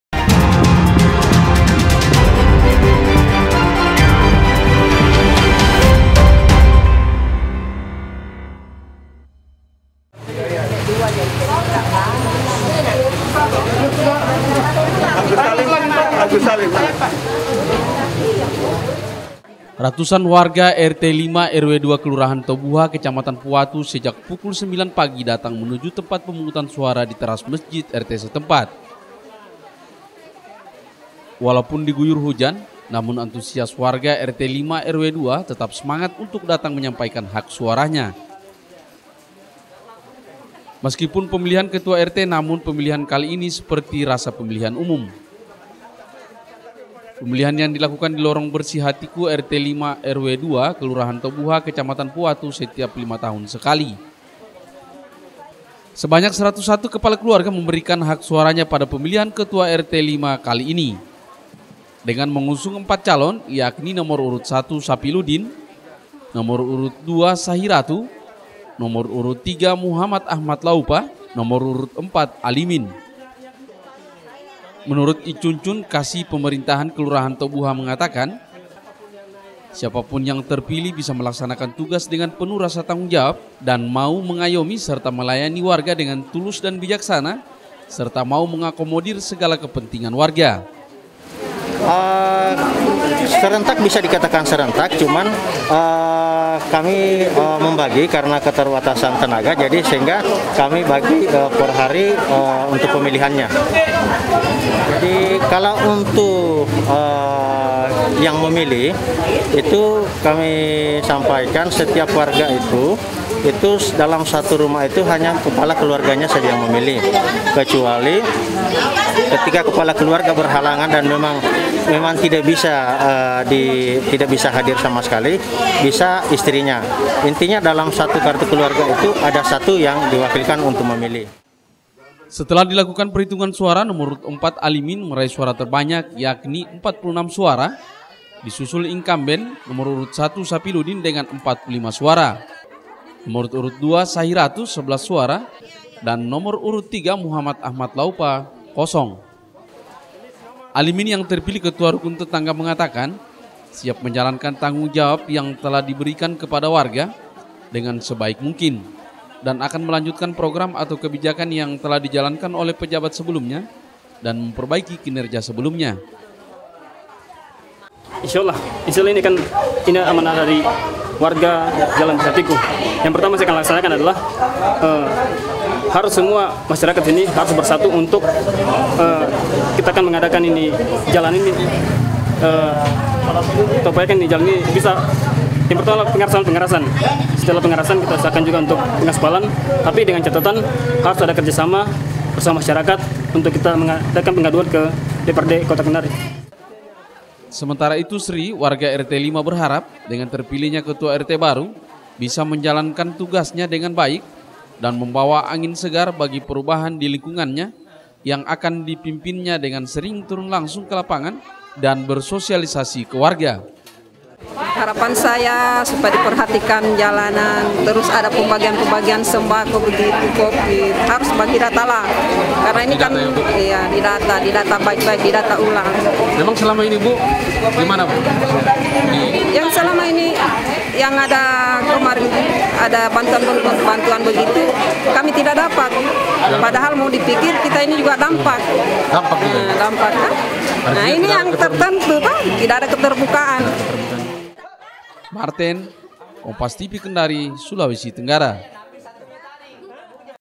Perolehan Suara Pemilihan Ketua RT 05 Selisih Satu
KENDARI, KOMPAS. TV - Ratusan warga rt 05 rw 02 kelurahan tobuuha kecamatan puuwatu, sejak pukul sembilan pagi datang menuju tempat pemungutan suara di teras mesjid rt setempat.

walaupun di guyur hujan namun antusias warga rt 05 rw 02 tetap semangat untuk datang  menyampaikan hak suaranya.

meskipun pemelihan ketua rt namun pemilihan kali ini seperti rasa pemilihan umum .

sebanyak seratus satu kepala keluarga memberikan hak suaranya pada pemilihan ketua rt 05 kali ini, dengan mengusung 4 calon yakni nomor urut 1. Sapiluddin,  2. Sahiratu,  3. muhammad ahmad laupa, 4. Alimin.